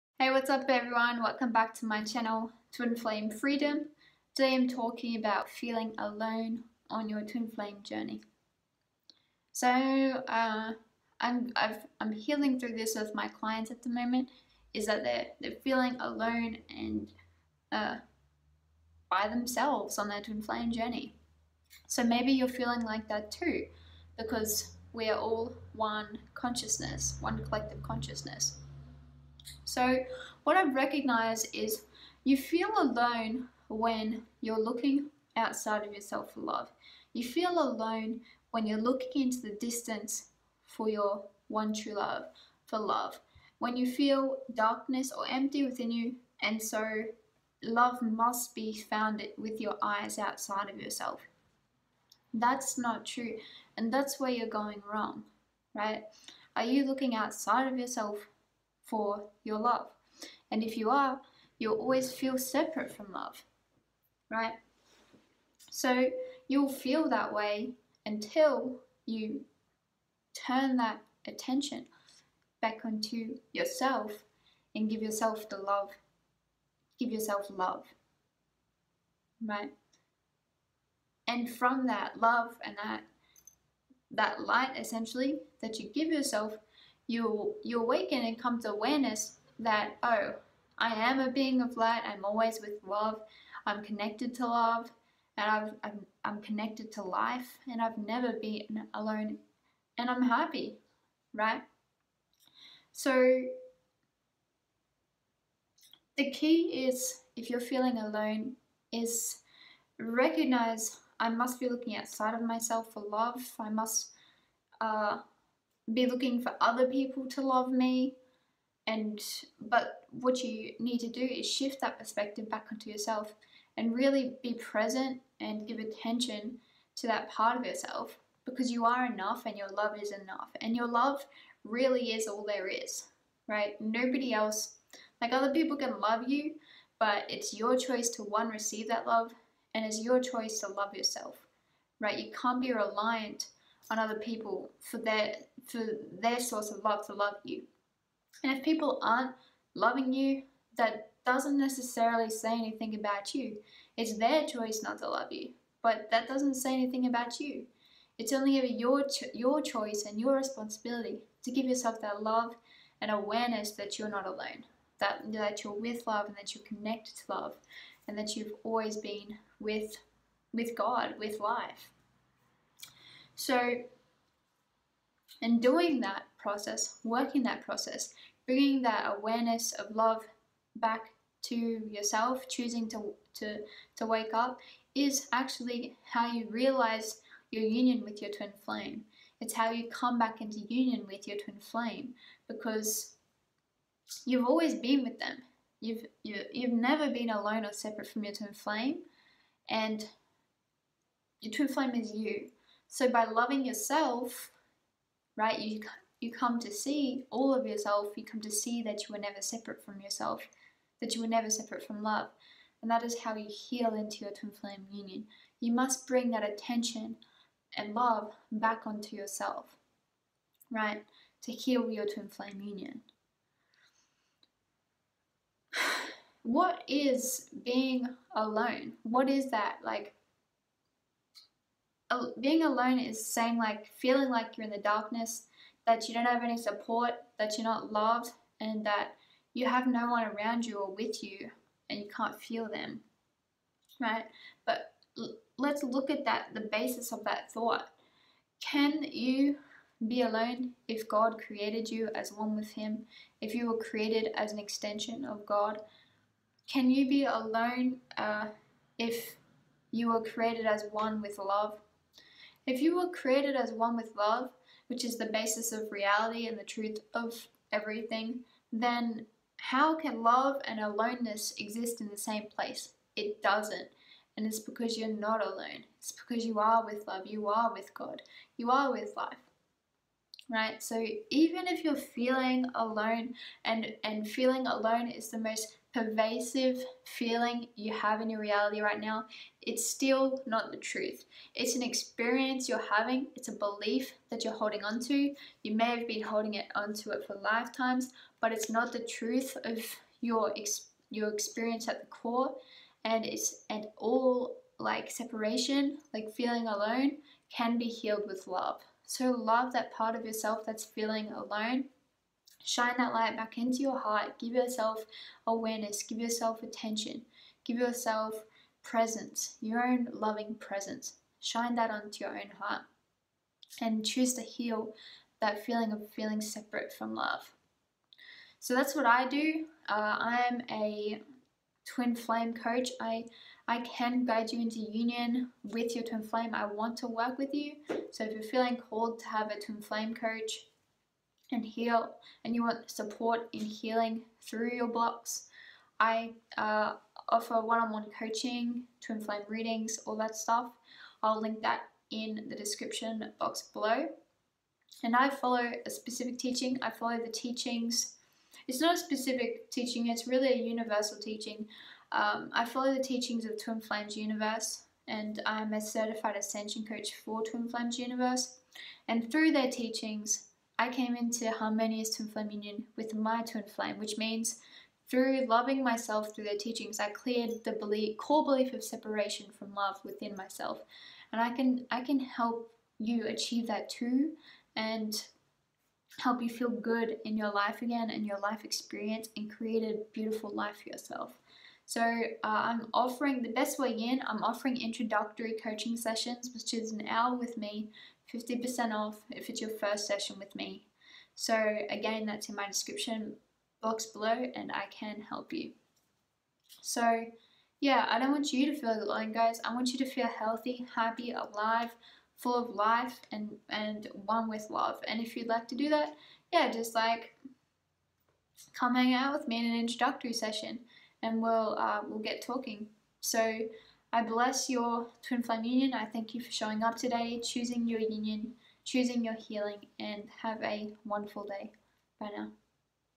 🔥 Feeling lonely on the Twin Flame Journey?? 🔥 Healing loneliness for good! 💏 ❤️
There is only 1 reason why you could feel lonely on the Twin Flame journey! and here is how to heal loneliness for good!

💙 %50 Off Introductory Coaching Session!